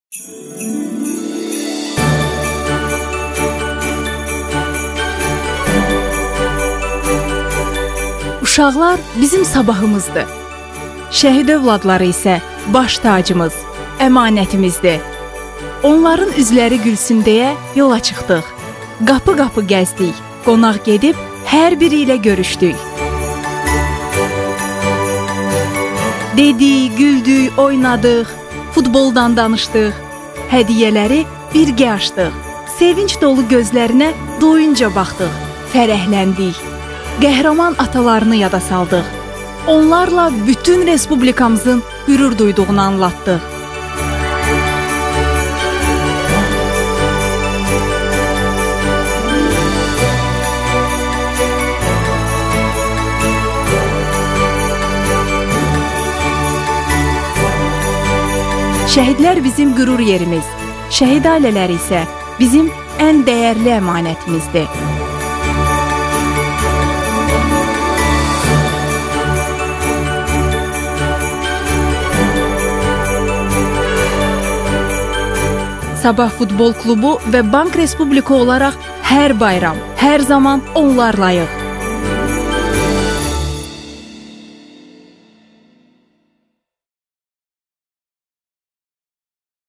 Bank Respublika və "Sabah" FK Yeni il öncəsi şəhid övladlarını sevindirdi!
Futbol üzrə ölkə çempionatının liderlərindən biri olan “Sabah” FK və klubun baş sponsoru olduğu ölkənin aparıcı banklarından biri olan Bank Respublika Yeni il bayramı ərəfəsində təqdirəlayiq aksiya həyata keçiriblər.
Belə ki, aksiya çərçivəsində komandanın həm yerli həm əcnəbi futbolçuları Bank əməkdaşları ilə birgə klubun yerləşdiyi Masazır qəsəbəsində məskunlaşmış şəhid ailələrini ziyarət edib.
Ziyarət zamanı futbolçular və Bankın nümayəndələri Vətənimizin bütövlüyü uğrunda canlarından keçmiş şəhidlərimizin övladlarına müxtəlif hədiyyələr təqdim edərək bayram sevinci yaşadıblar.
Görüş zamanı şəhid övladlarına atalarının qəhrəmanlıqları ilə daim fəxr etməli olduqları, onların Vətənimiz üçün etdiklərinin heç vaxt unudulmayacağı qeyd edilib.